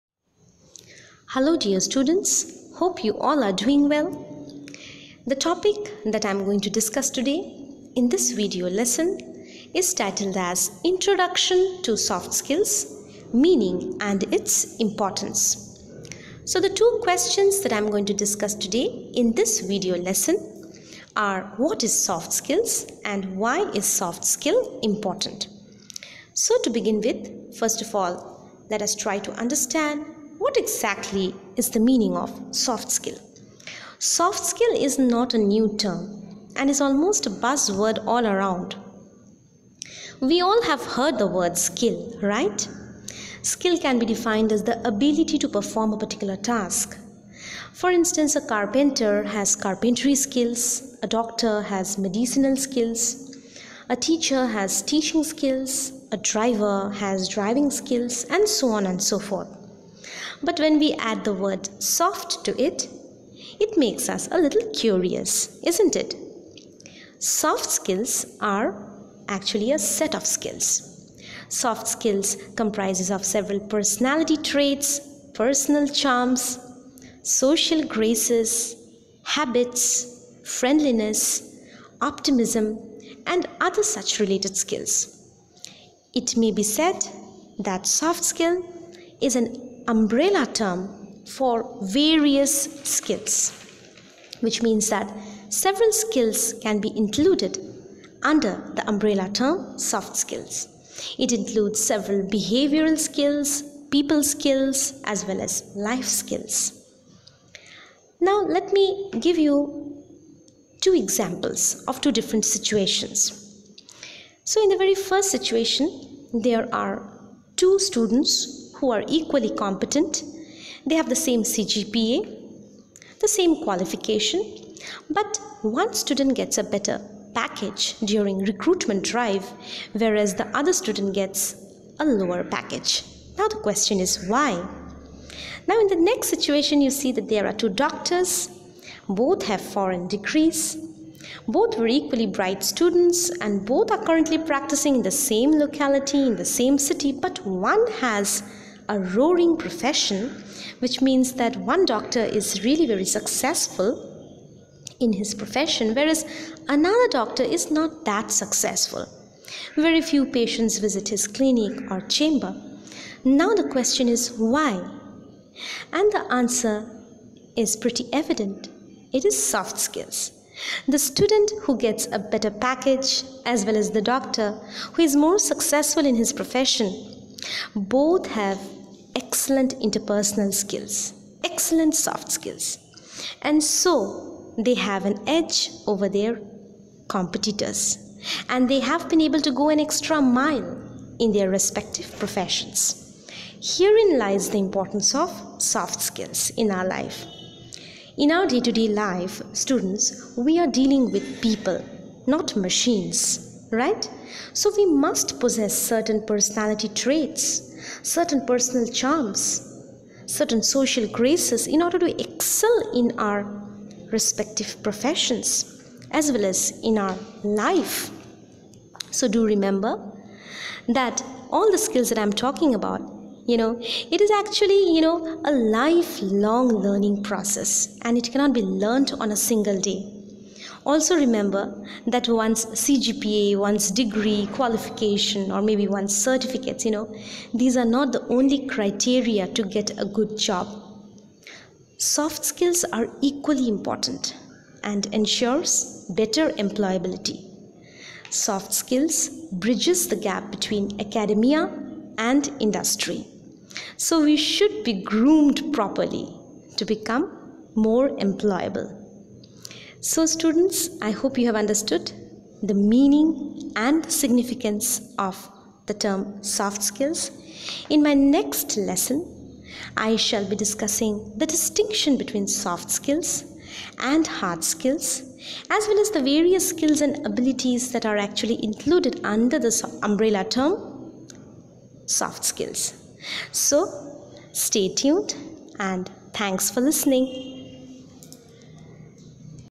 Introduction to Soft Skills (Meaning & Importance) II Nabamita Das
This video lesson shall enable the students to understand the meaning of the term 'Soft Skills' as well as its importance in our profession and life. This video lesson discusses in detail that soft skills are actually a set of skills. It comprises of several personality traits, personal charms, social graces, habits, friendliness and optimism. It includes various behavioural skills, people skills as well as life skills. This video lesson  shall also help the learners to understand with the help of examples how soft skills ensures better employability and bridges the gap between academia and industry.